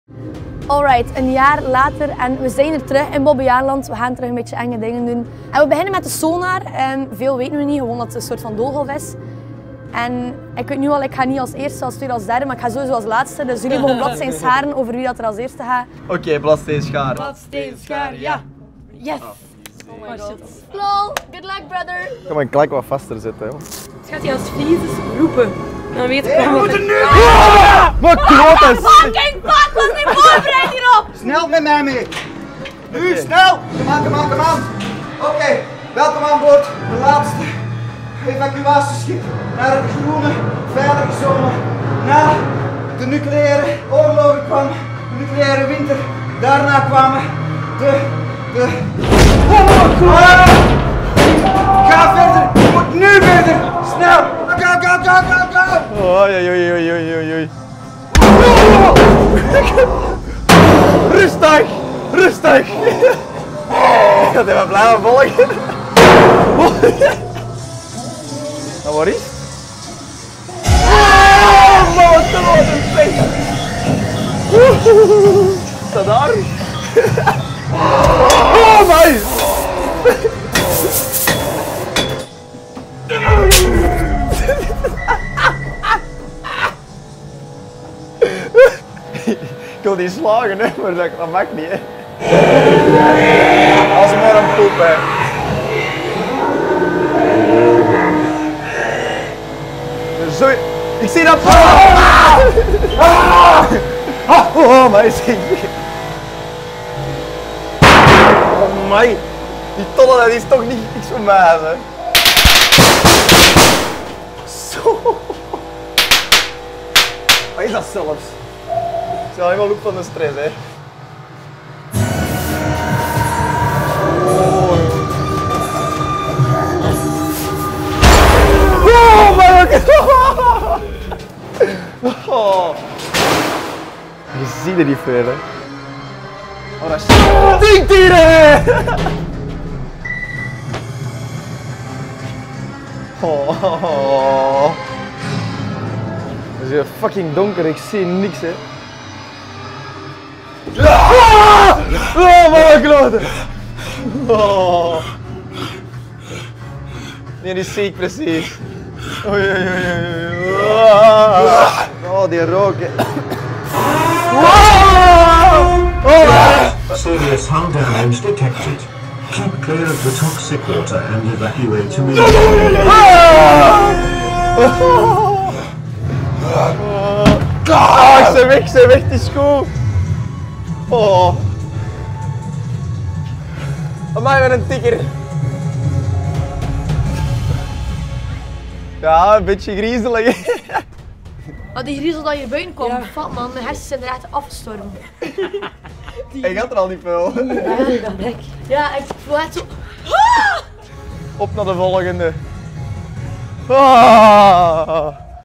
Halloween 2018 🎃 5NAPBACK in de 'Sonar'
5NAPBACK moet alleen door de scare maze 'Sonar' gaan in Bobbejaanland. Kan hij het aan?